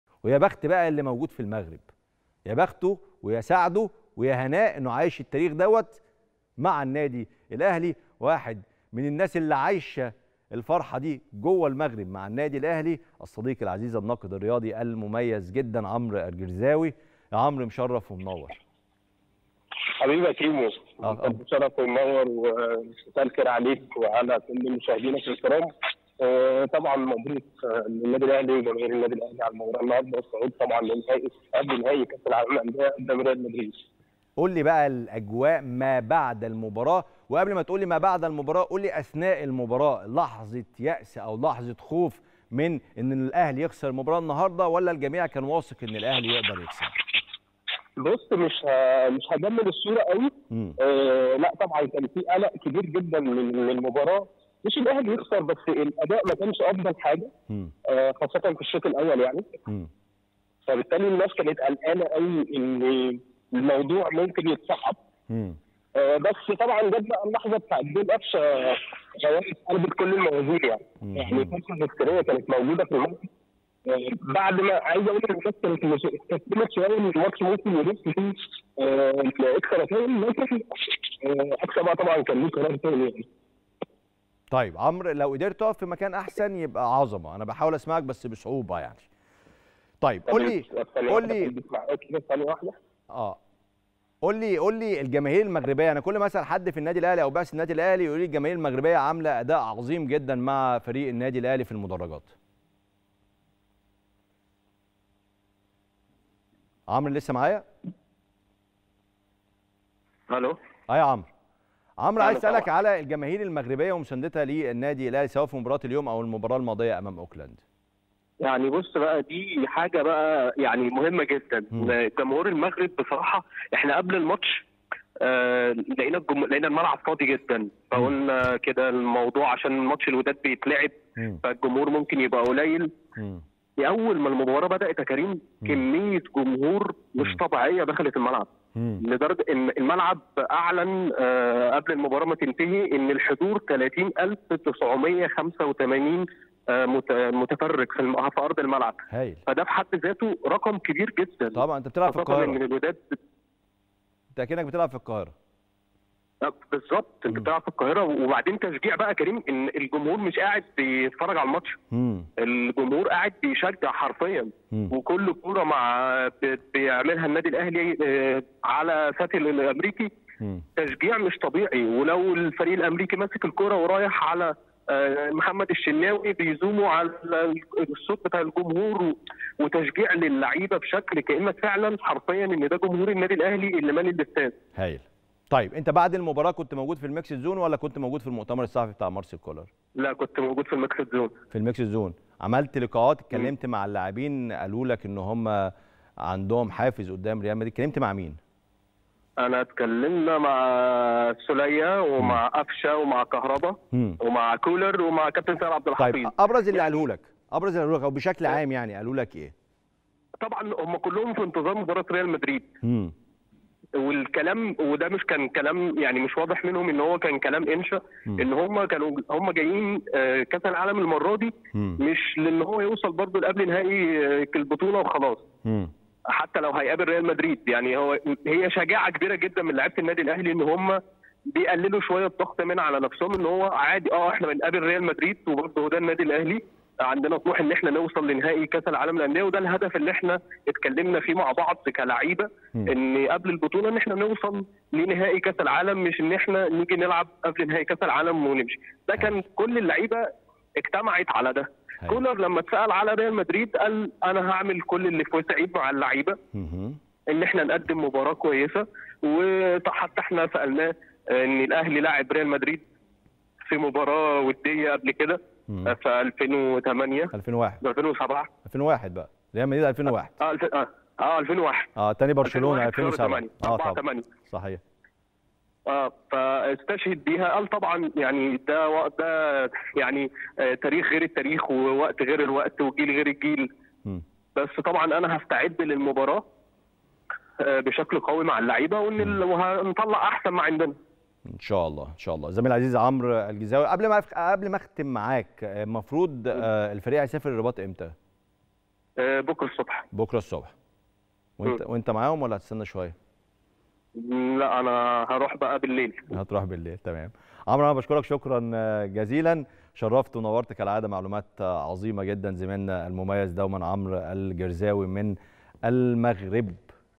"كان في كمية جمهور مغربي دخلت أرض الملعب بطريقة كبيرة" .. عمرو الجزراوي: كنت حاسس ان في مصر
💥💥"كان في كمية جمهور مغربي دخلت أرض الملعب بطريقة كبيرة .. وكان التشجيع مش طبيعي" .. الناقد الرياضي عمرو الجزراوي: كنت حاسس ان في مصر 🔥🔥ويكشف الكواليس ما بعد مباراة الاهلي وسياتل الامريكي 💣👇🏻